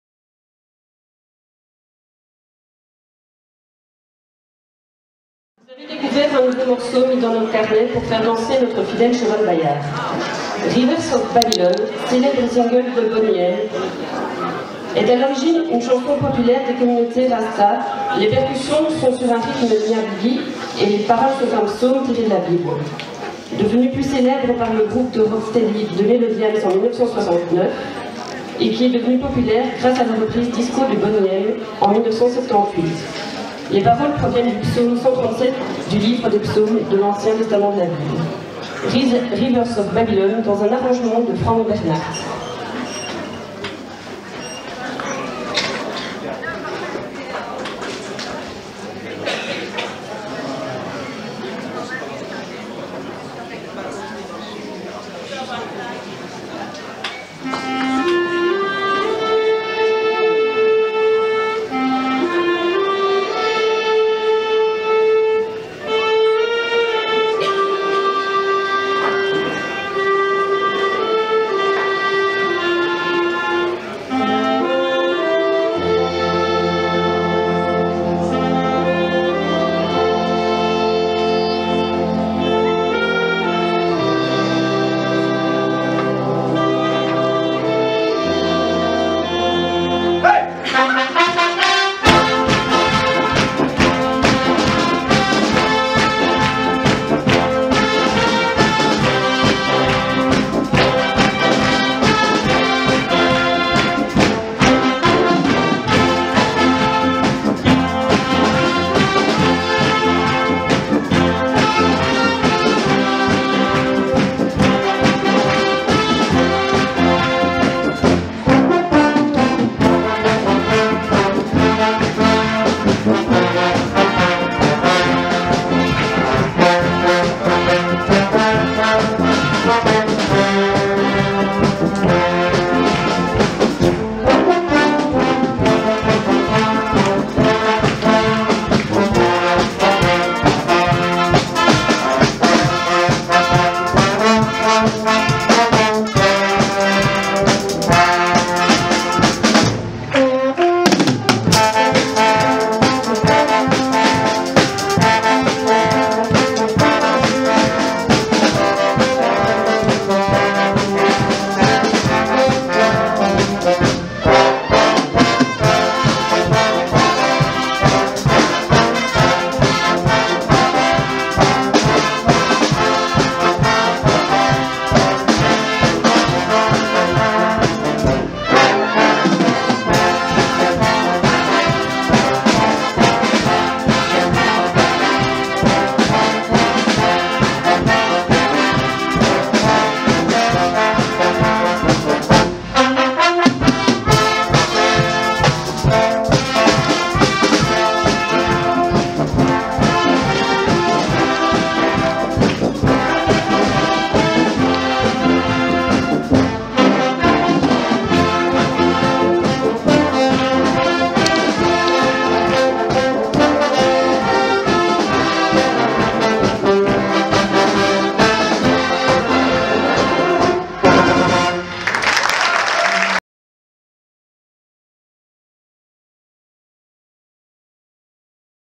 Royale Fanfare Communale Huissignies
Extrait du concert - Cabaret à Huissignies.
17 mars 2019
Vidéo : Denis k